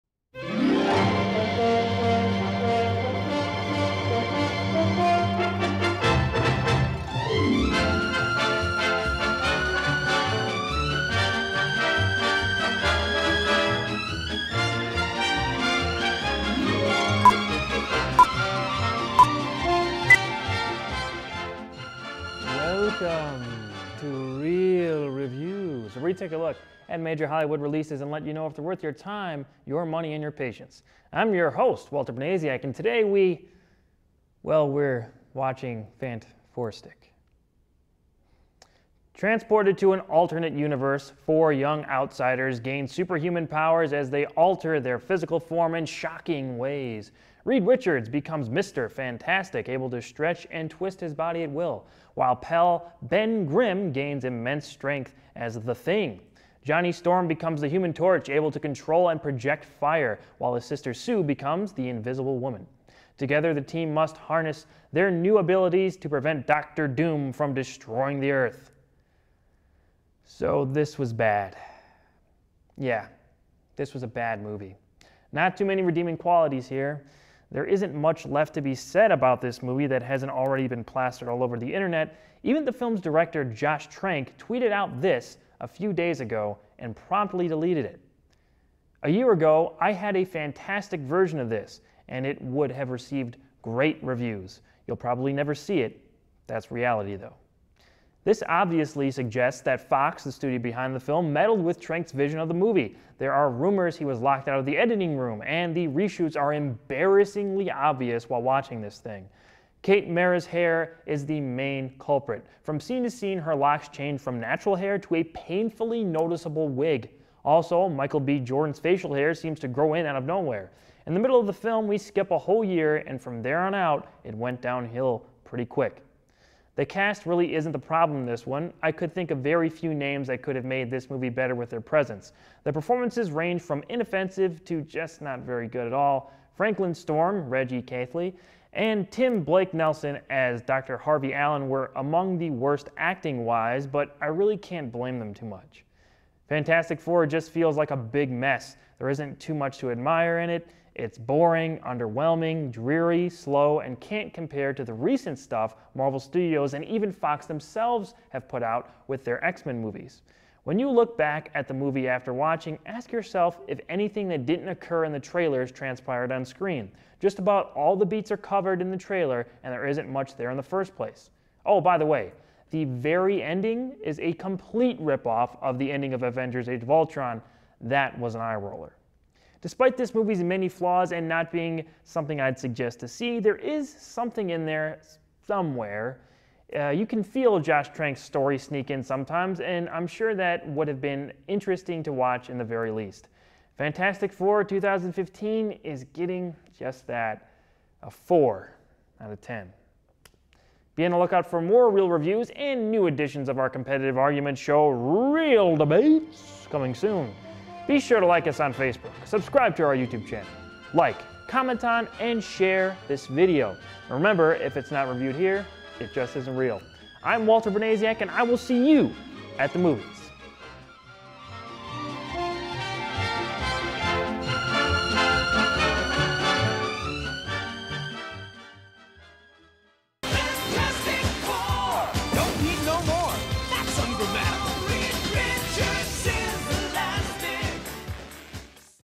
Reel Reviews - "Fantastic Four" (2015)
Walter looks at this years most unfantastic flop...

code10studios.net